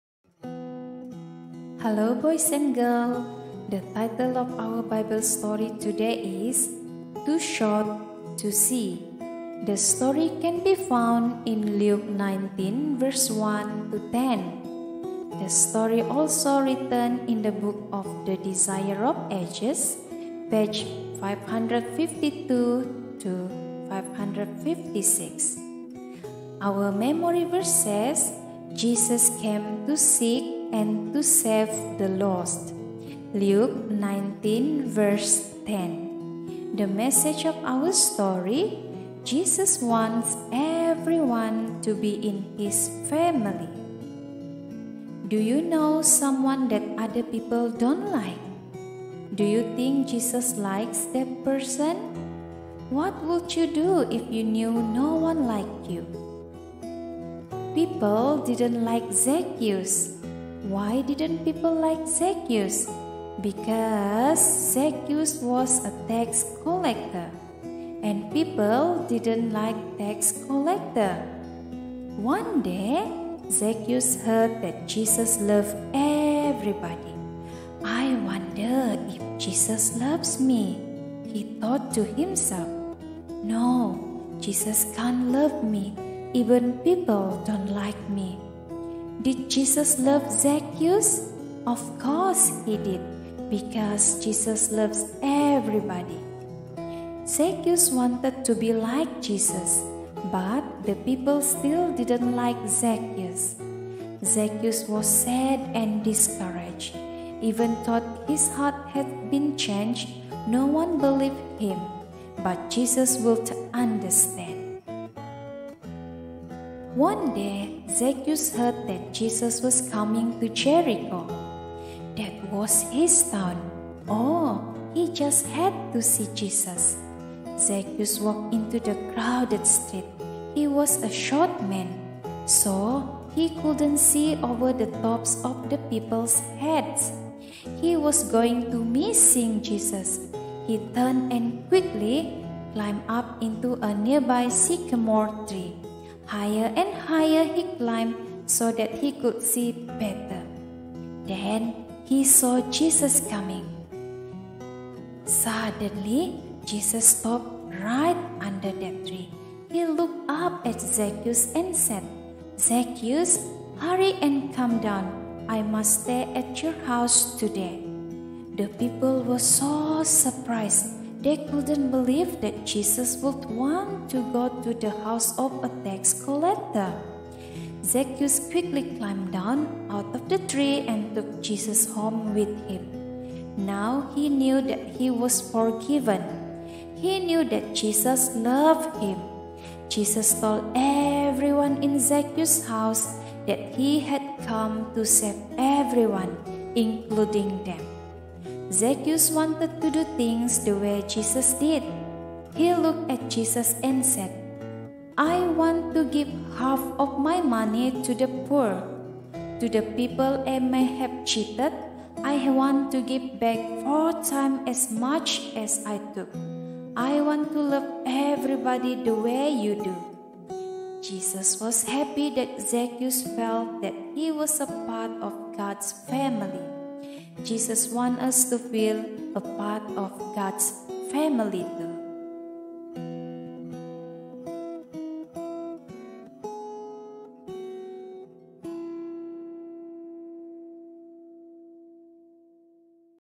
"Too Short To See" - Kindergarten Sabbath School Story, Lesson 6, Q1 2021 - Bible Story for Children
Bible Story for Children made by Me and My Husband for our little James (3YO). Story inspired from Quarterly Kindergarten Sabbath School Lesson.

Story read by Mummy James
Video edited by Daddy James